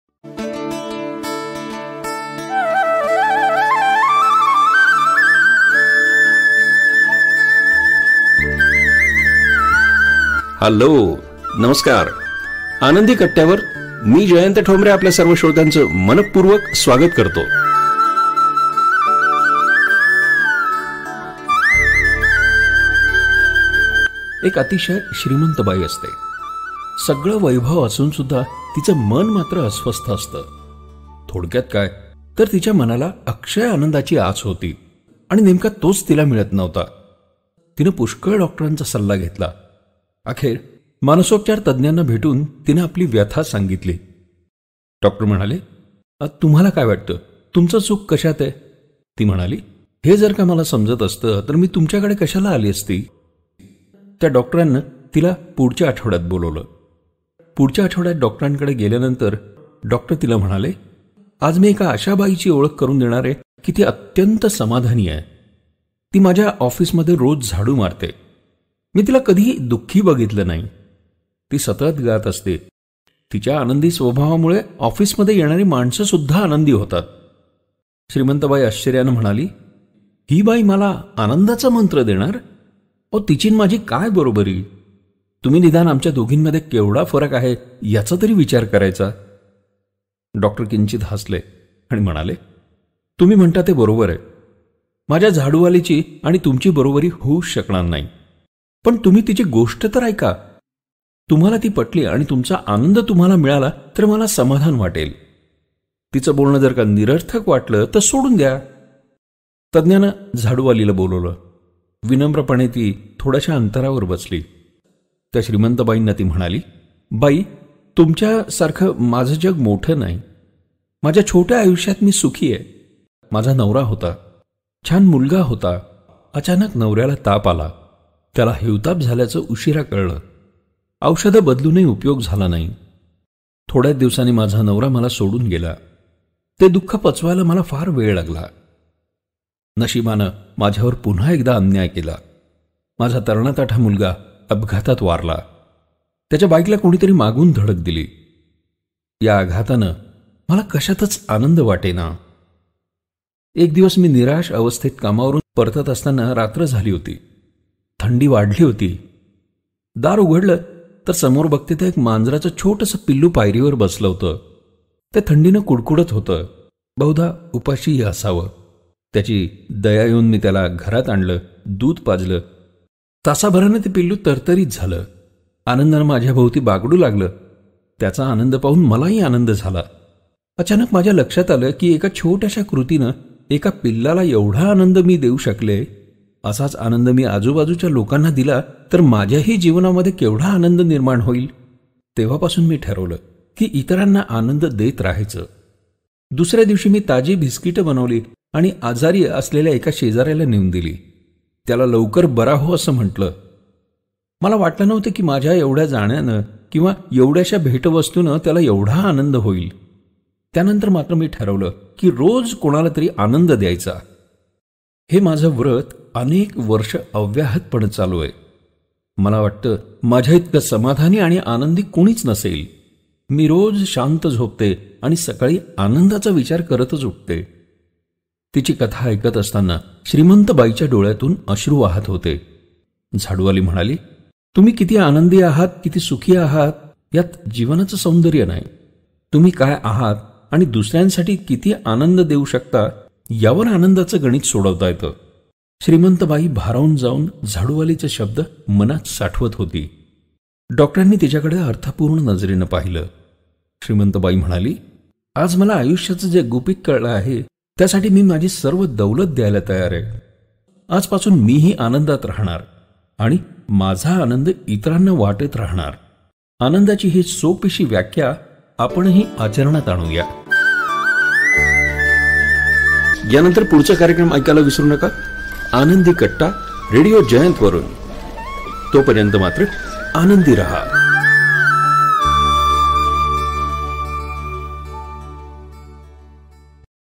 Anandi Katta Season - 1 Episode - 33 (Ananda)
स्वतःच दुःख विसरून दुसऱ्याला अगदी छोट्या छोट्या गोष्टींमधून दिलेला आनंद खूप समाधान देऊन जातो . कसं ? ऐकुया आजच्या आनंदी कट्ट्यावर ..